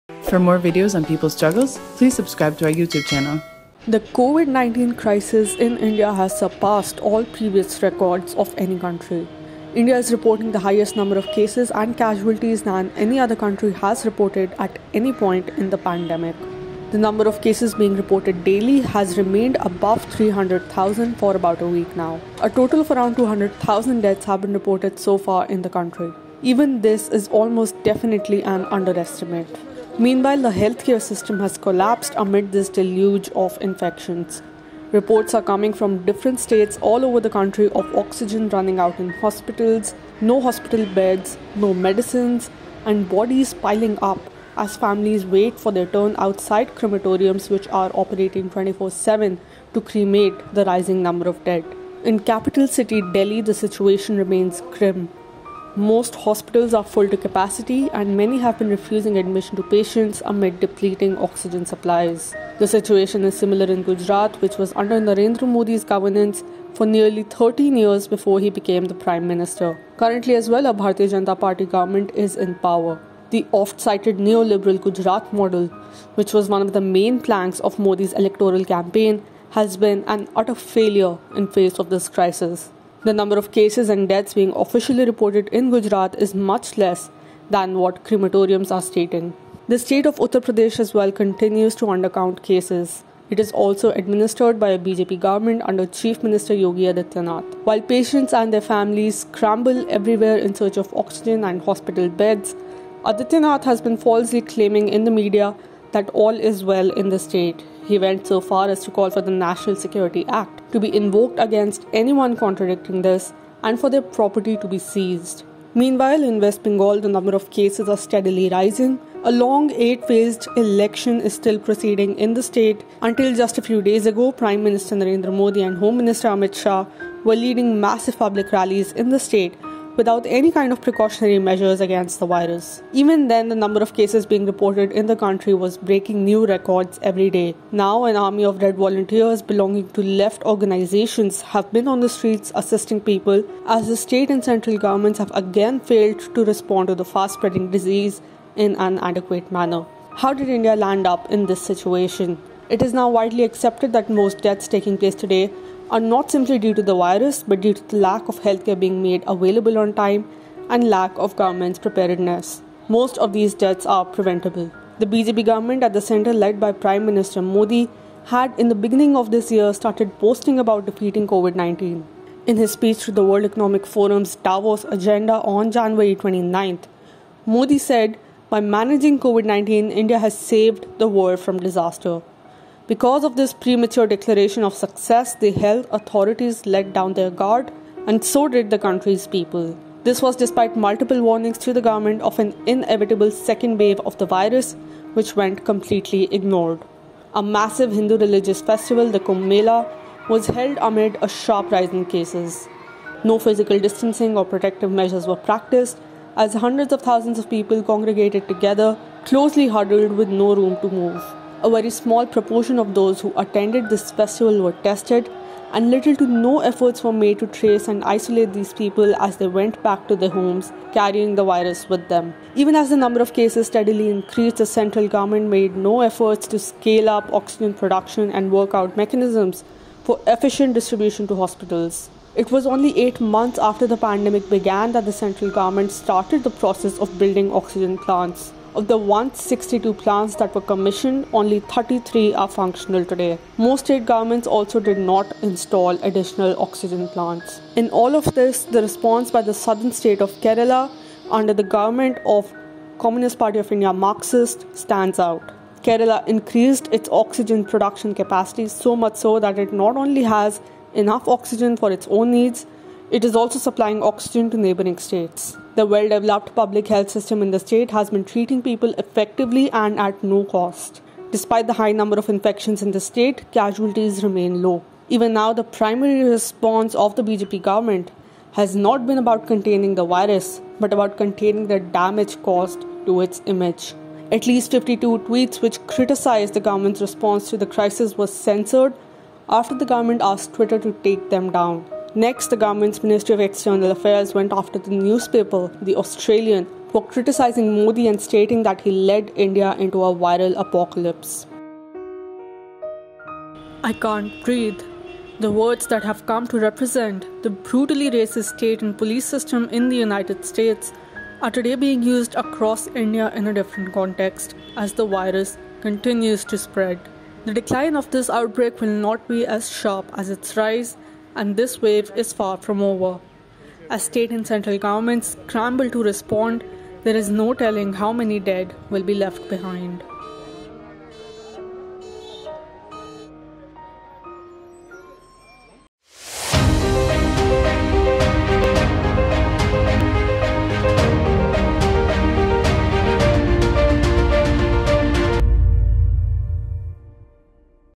COVID-19 crisis in India: A complete systemic collapse
The COVID-19 crisis in India has surpassed all previous records of any country. India is reporting the highest number of cases and casualties than any other country has reported at any point in the pandemic. Meanwhile, the healthcare system has collapsed amid this deluge of infections. Reports are coming from different states all over the country of oxygen running out in hospitals, no hospital beds, no medicines, and bodies piling up as families wait for their turn outside crematoriums which are operating 24/7 to cremate the rising number of dead.